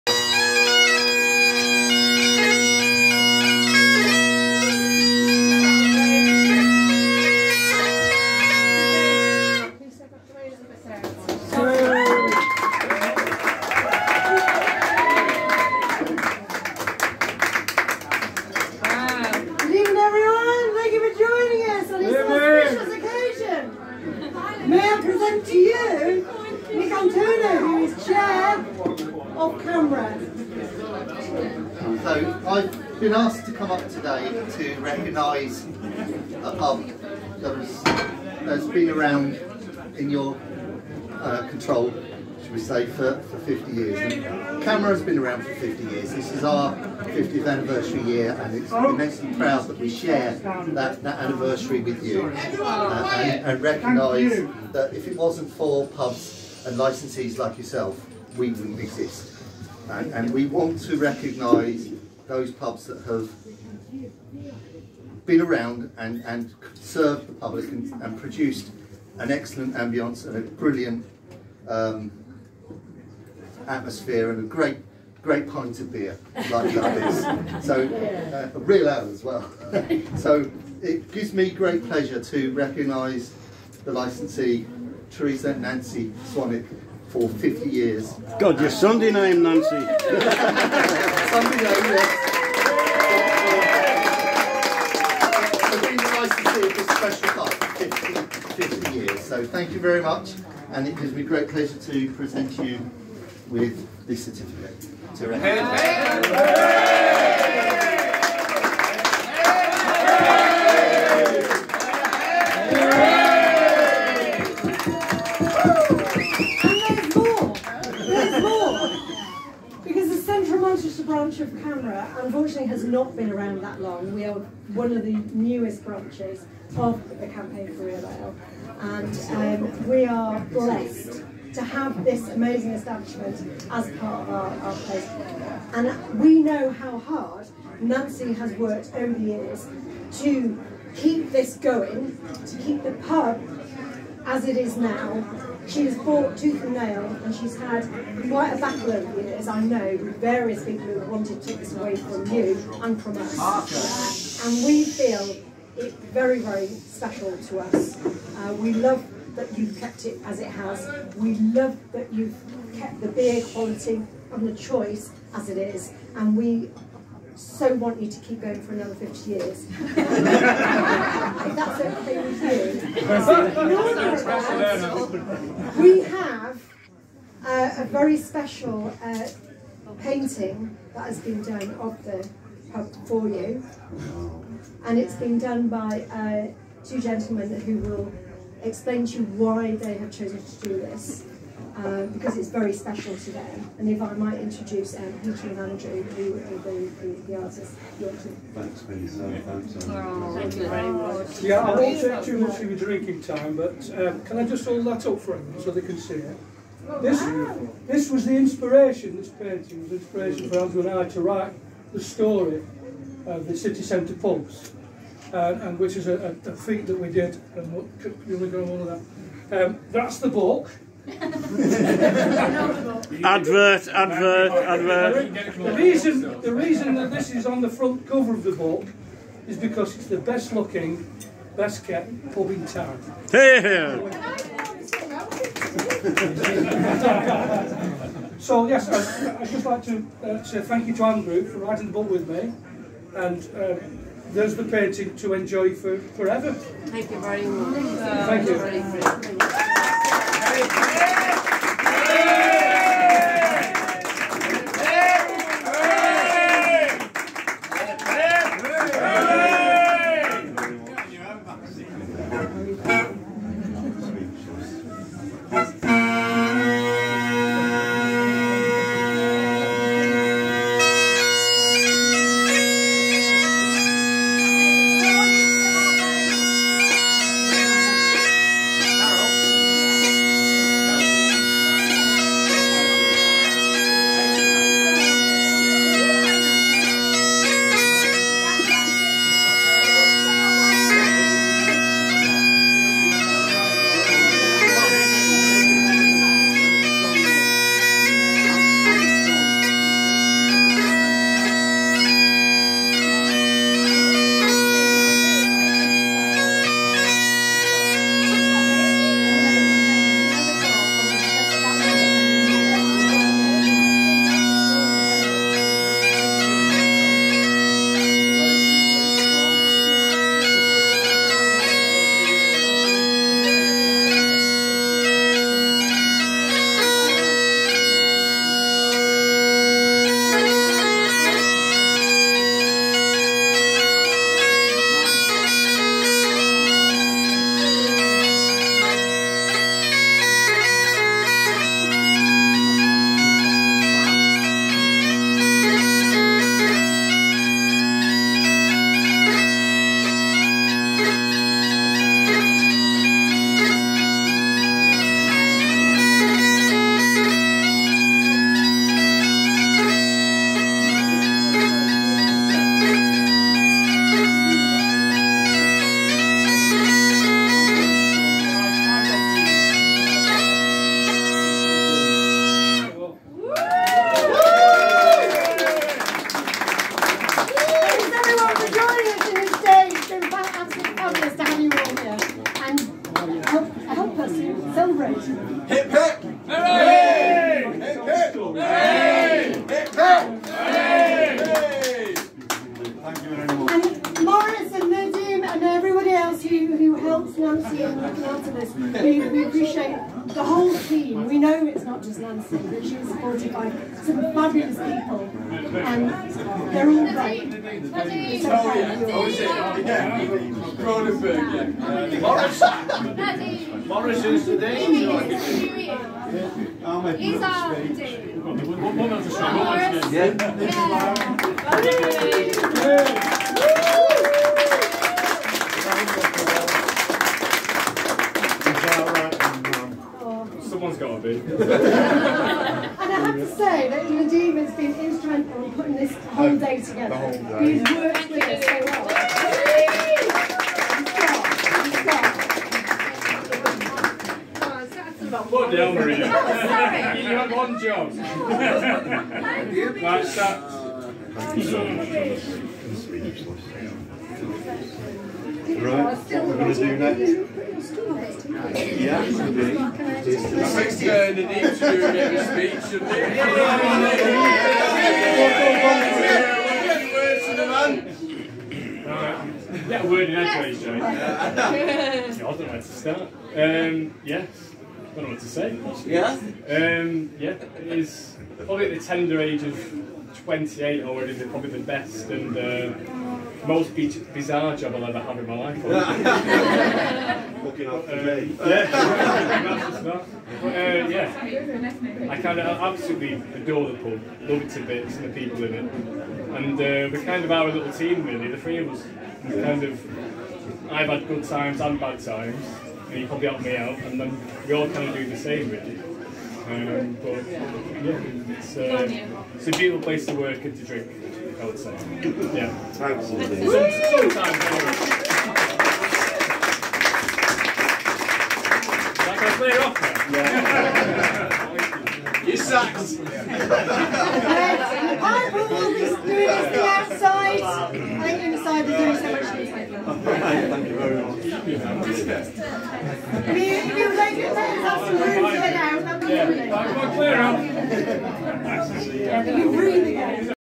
Peveril Of The Peak 50 Years
Presentation to Nancy, landlady of the Peveril Of The Peak, Manchester to celebrate 50 years as the licensee of the pub.